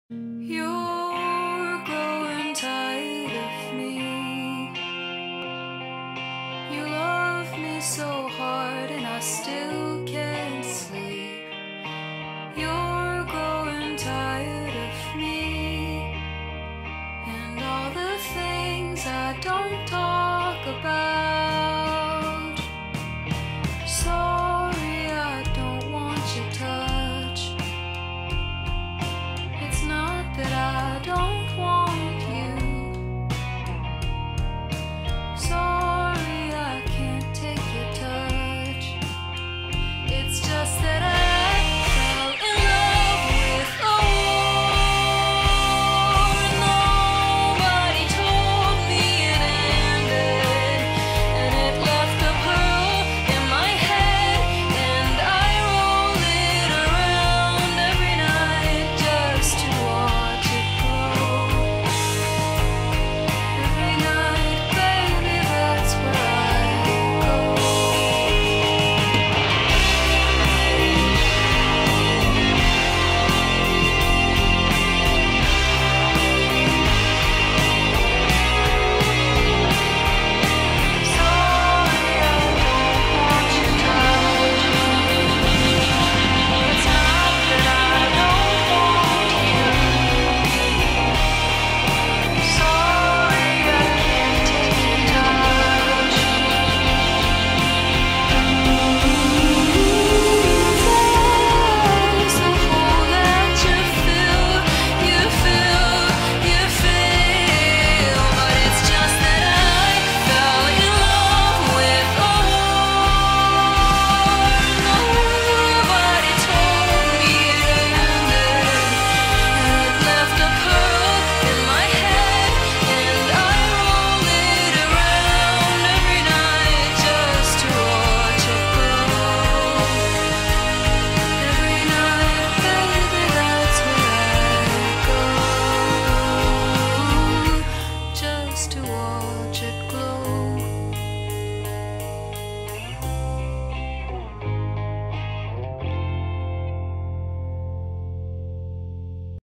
a pink prom pearl
[flash warning] the night i heard this song, it knew was something. the beauty of the environment and my friends is something i love, it makes me see how beautiful they are.